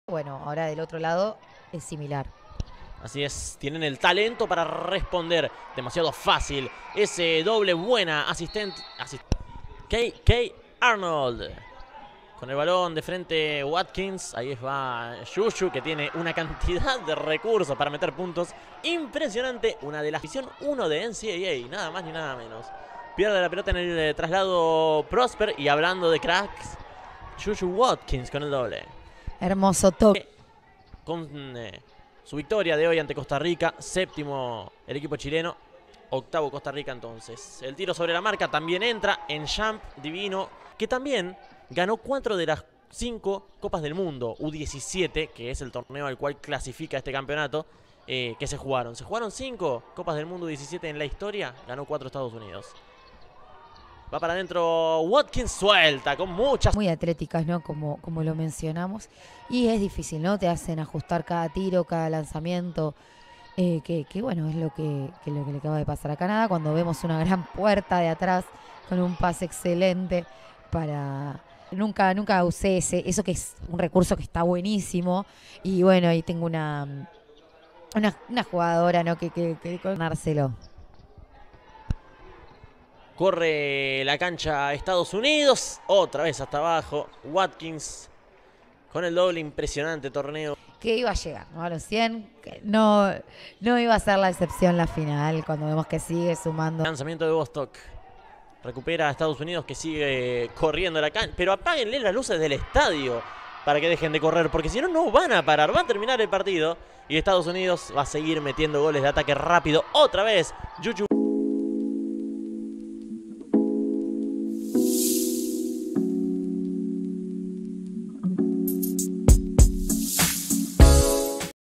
FIBAU16 | JuJu Watkings vs Canadá | FINAL
Estados  Umidos se quedó con el primer lugar en el Premundial FIBA Americas U16 luego de vencer a Canadá 118-45 con Juju Watkins como la máxima anotadora con 28 puntos en 18 minutos.